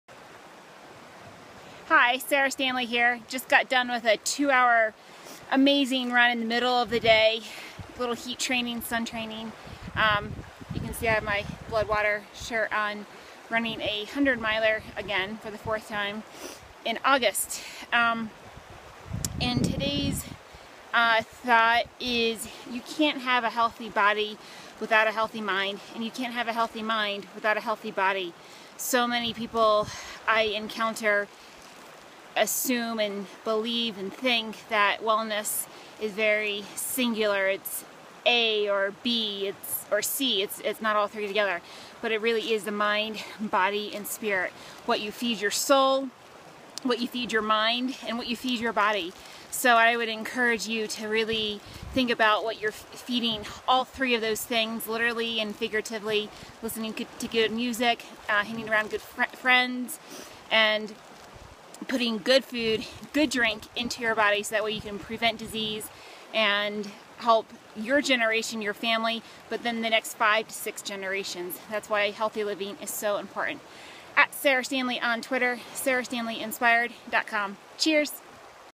Healthy mind for a healthy body
100 miler training update and a thought about healthy minds and bodies.